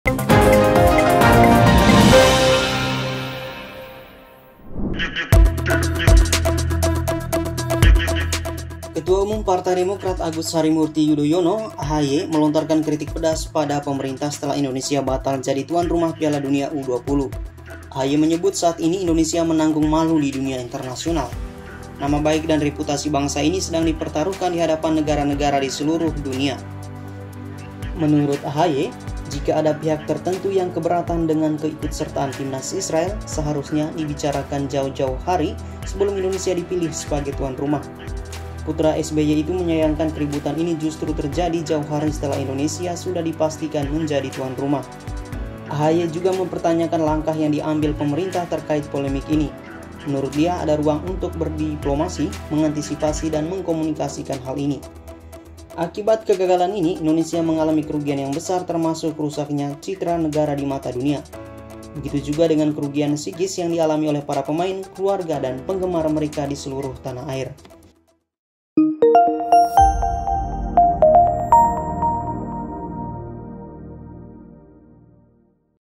Kritik Pedas AHY Saat Indonesia Batal Jadi Tuan Rumah Piala Dunia U20
Ketua Umum Partai Demokrat Agus Harimurti Yudhoyono melontarkan kritik pedas pada pemerintah setelah Indonesia batal jadi tuan rumah Piala Dunia U-20.